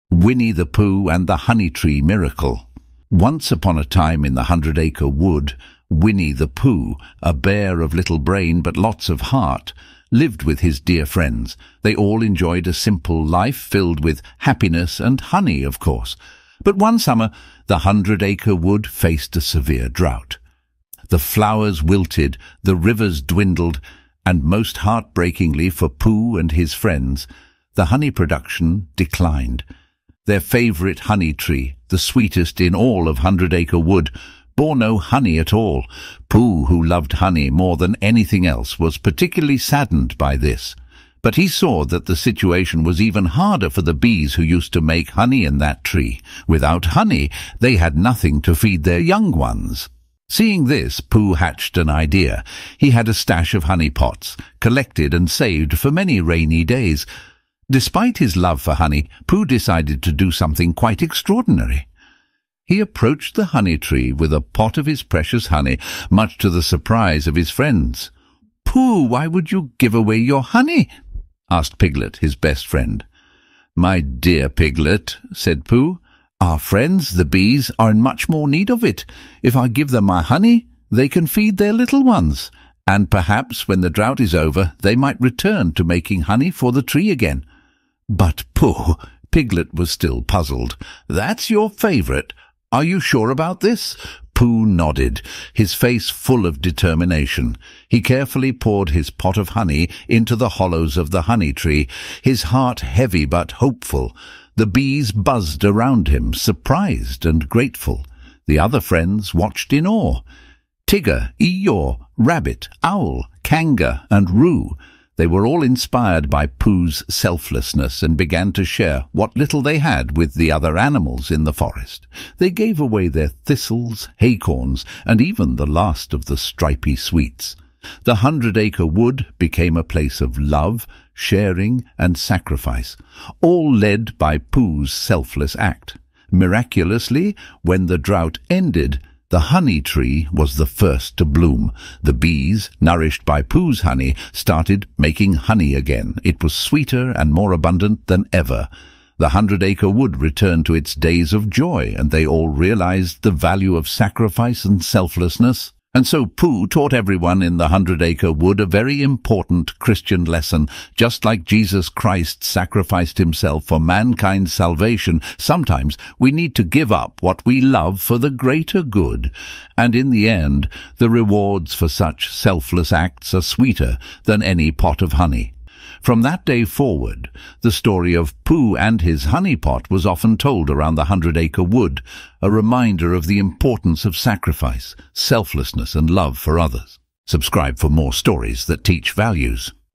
Christian Adaptation of Winnie the Pooh | Audiobook | Stories for Kids
Christian Adaptation of Winnie the Pooh | Audio Book Stories for Kids.

"Winnie the Pooh and the Honey Tree Miracle"

Once upon a time in the Hundred Acre Wood, Winnie the Pooh, a bear of little brain but lots of heart, lived with his dear friends. They all enjoyed a simple life filled with happiness and honey, of course. But one summer, the Hundred Acre Wood faced a severe drought. The flowers wilted, the rivers dwindled, and most heartbreakingly for Pooh and his friends, the honey production declined.

Their favorite honey tree, the sweetest in all of Hundred Acre Wood, bore no honey at all. Pooh, who loved honey more than anything else, was particularly saddened by this. But he saw that the situation was even harder for the bees who used to make honey in that tree. Without honey, they had nothing to feed their young ones.

Seeing this, Pooh hatched an idea. He had a stash of honey pots, collected and saved for many rainy days. Despite his love for honey, Pooh decided to do something quite extraordinary. He approached the honey tree with a pot of his precious honey, much to the surprise of his friends.

"Pooh, why would you give away your honey?" asked Piglet, his best friend.

"My dear Piglet," said Pooh, "Our friends, the bees, are in much more need of it. If I give them my honey, they can feed their little ones. And perhaps when the drought is over, they might return to making honey for the tree again."

"But Pooh," Piglet was still puzzled, "that's your favorite! Are you sure about this?"

Pooh nodded, his face full of determination. He carefully poured his pot of honey into the hollows of the honey tree, his heart heavy but hopeful. The bees buzzed around him, surprised and grateful.

The other friends watched in awe. Tigger, Eeyore, Rabbit, Owl, Kanga, and Roo - they were all inspired by Pooh's selflessness and began to share what little they had with the other animals in the forest. They gave away their thistles, haycorns, and even the last of the stripy sweets. The Hundred Acre Wood became a place of love, sharing, and sacrifice, all led by Pooh's selfless act.

Miraculously, when the drought ended, the honey tree was the first to bloom. The bees, nourished by Pooh's honey, started making honey again. It was sweeter and more abundant than ever. The Hundred Acre Wood returned to its days of joy, and they all realized the value of sacrifice and selflessness.

And so, Pooh taught everyone in the Hundred Acre Wood a very important Christian lesson: just like Jesus Christ sacrificed himself for mankind's salvation, sometimes, we need to give up what we love for the greater good. And in the end, the rewards for such selfless acts are sweeter than any pot of honey.

From that day forward, the story of Pooh and his honey pot was often told around the Hundred Acre Wood, a reminder of the importance of sacrifice, selflessness, and love for others.